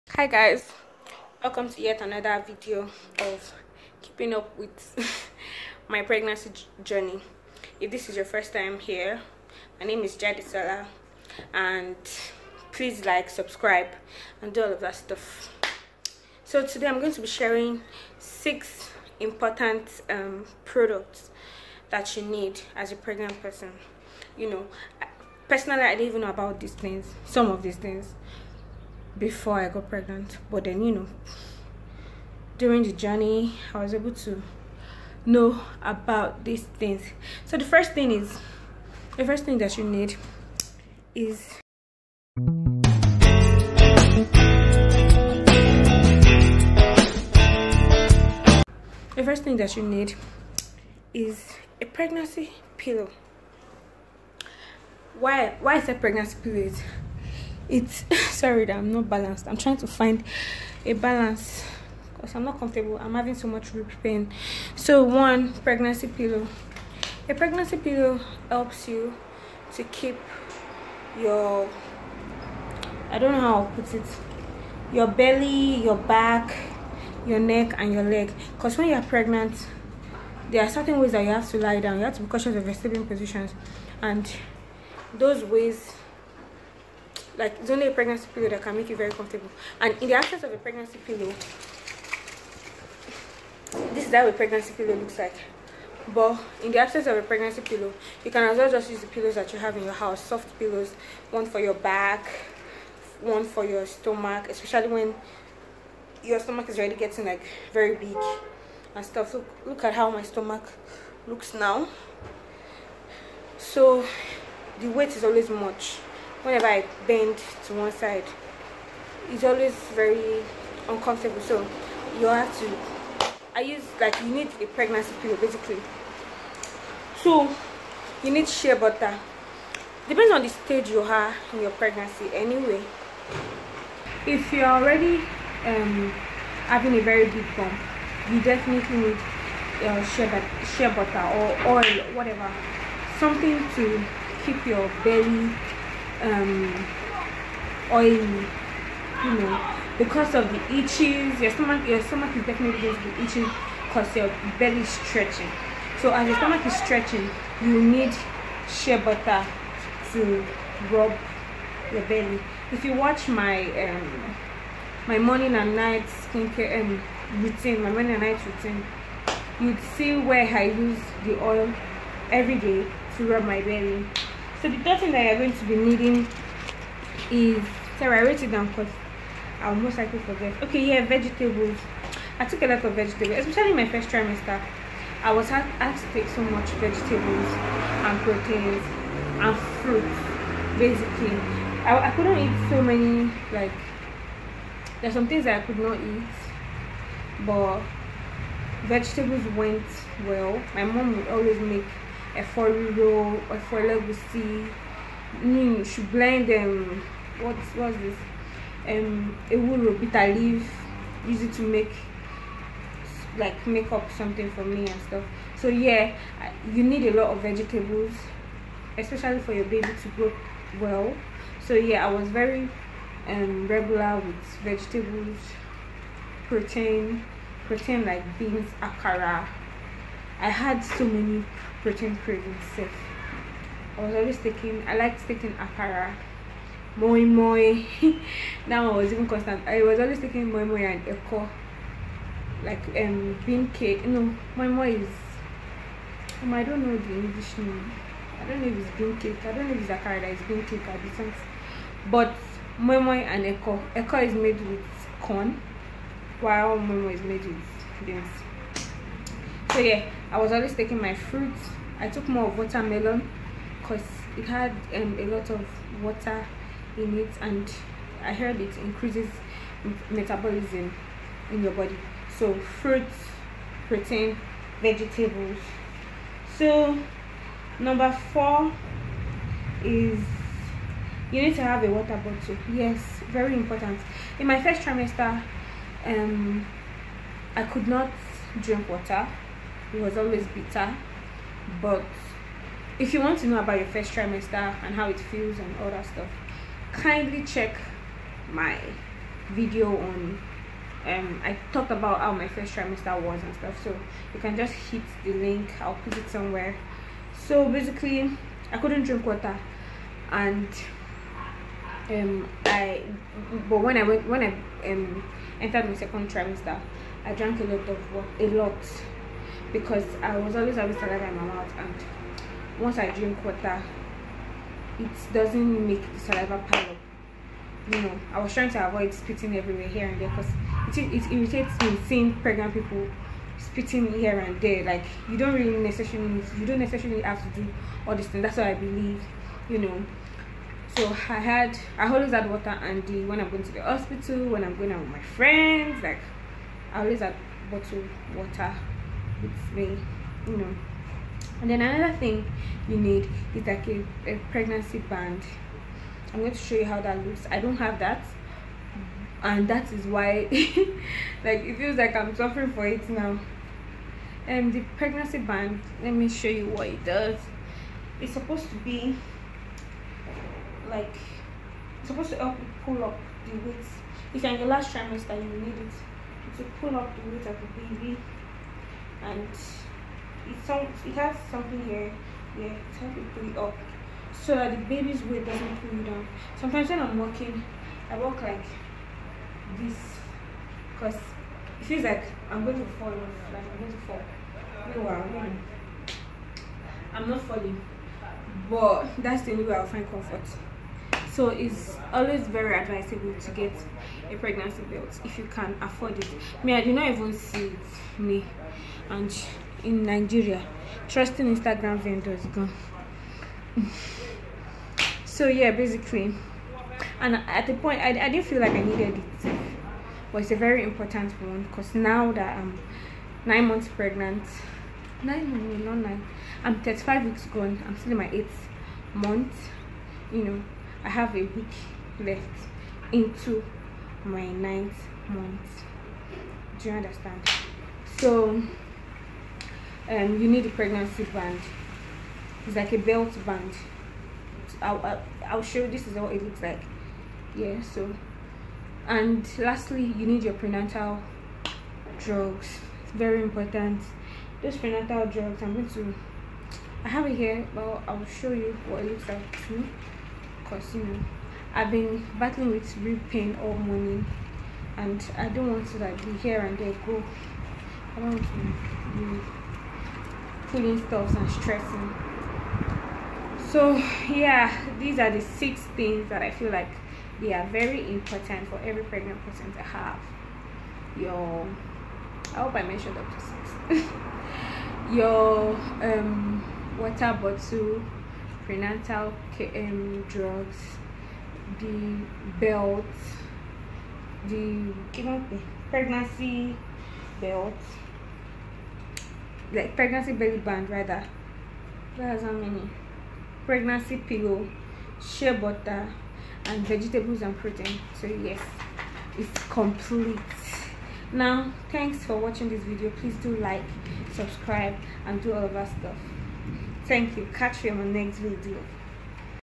PREGNANCY ESSENTIALS:What you need to have an easy pregnancy/relief items
Any video you see with this background wasnt filmed in my regular set-up space and I had to film it at the time. please ignore the background noise etc and focus on the message

As soon as you announce your pregnancy,you'll get plenty advices on products you'll be needing to have an easy ride. but how do you know what to listen to? you need to watch till the end to know which is more important and which can be improvised. The key thing is to know how these products can be useful for you

personal page: @Just_jade15